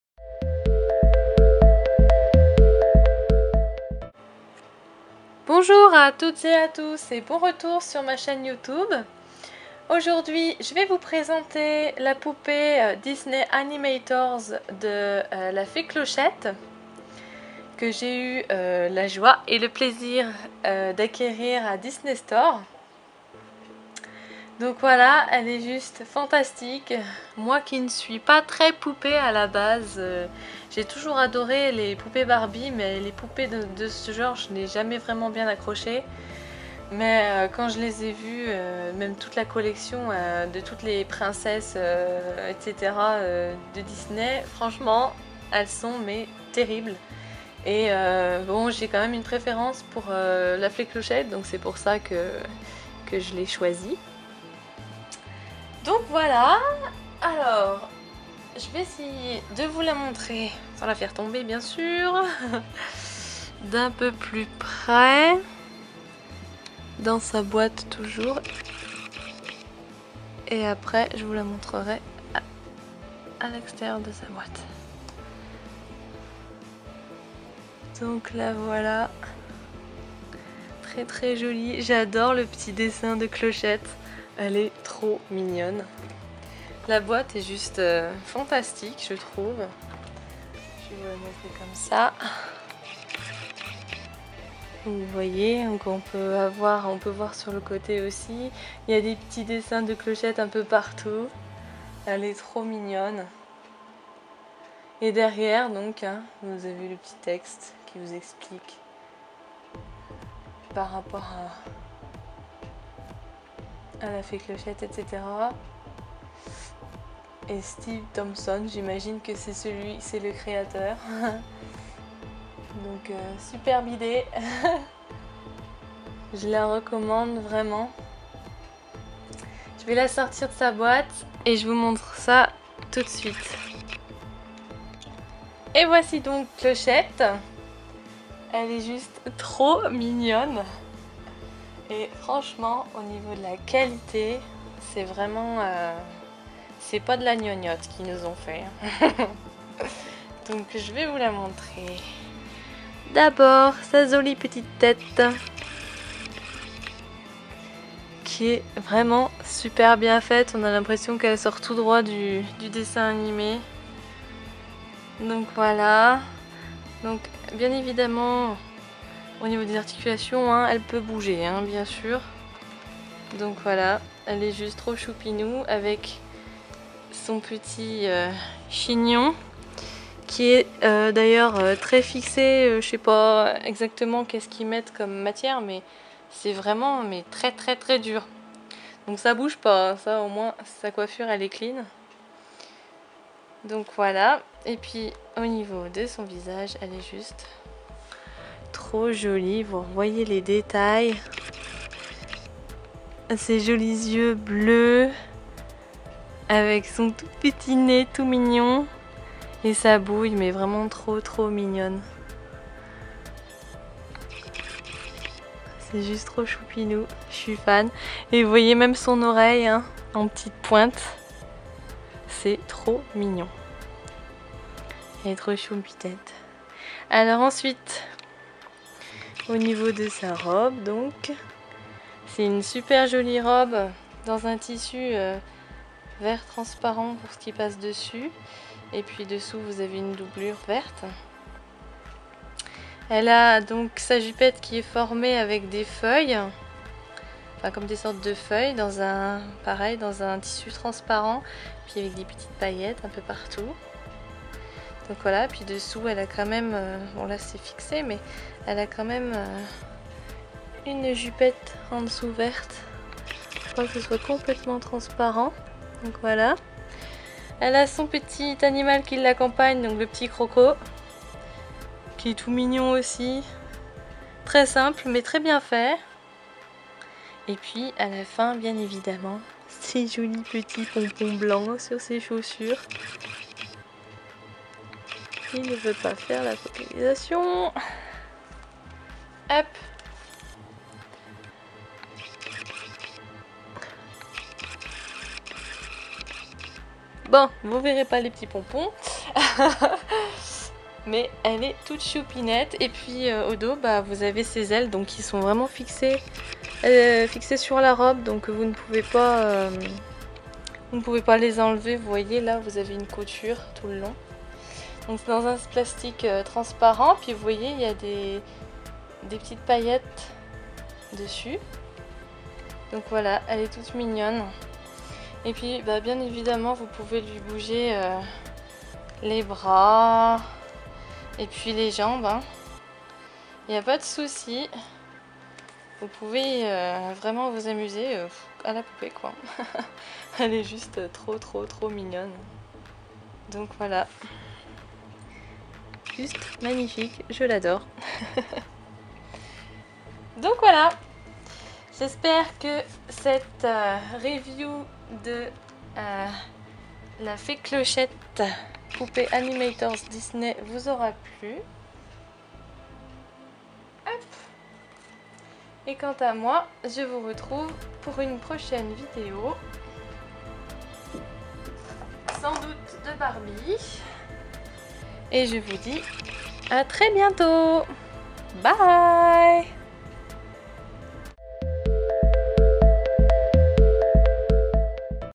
Fée Clochette Tinker Bell Disney Animators' doll review
Dans cette vidéo je vous présente la superbe poupée animators' de la fée clochette (disney).